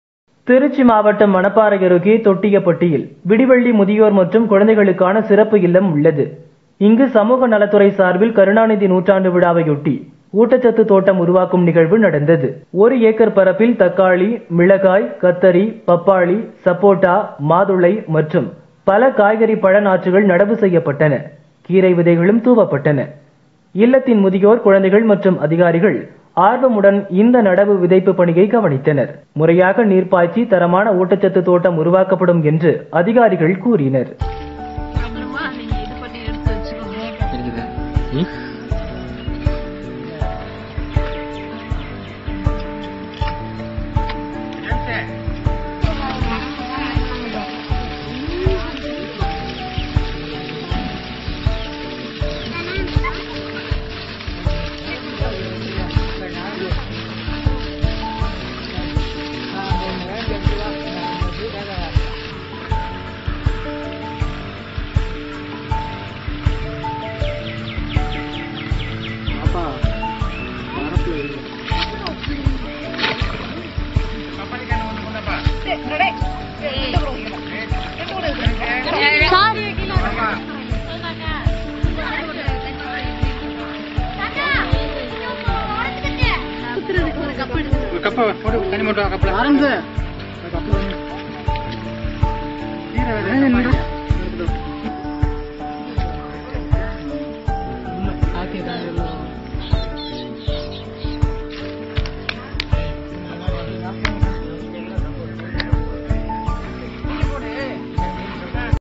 திருச்சியில் 1 ஏக்கரில் உருவாகும் பிரமாண்ட ஊட்டச்சத்து தோட்டம் | Trichy social welfare department
திருச்சி மாவட்டம் மணப்பாறை அருகே தொட்டியபட்டியில் விடிவெள்ளி முதியோர் மற்றும் குழந்தைகளுக்கான சிறப்பு இல்லம் உள்ளது.

இங்கு சமூக நலத்துறை சார்பில் கருணாநிதி நூற்றாண்டு விழாவையொட்டி ஊட்டச்சத்து தோட்டம் உருவாக்கும் நிகழ்வு நடந்தது.

ஒரு ஏக்கர் பரப்பில் தக்காளி, மிளகாய், கத்தரி, பப்பாளி, சப்போட்டா, மாதுளை மற்றும் பல காய்கறி, பழ நாற்றுகள் நடவு செய்யப்பட்டன. கீரை விதைகளும் தூவப்பட்டன.

இல்லத்தின் முதியோர், குழந்தைகள் மற்றும் அதிகாரிகள் ஆர்வமுடன் இந்த நடவு, விதைப்பு பணியை கவனித்தனர். முறையாக நீர் பாய்ச்சி தரமான ஊட்டச்சத்து தோட்டம் உருவாக்கப்படும் என்று அதிகாரிகள் கூறினர்.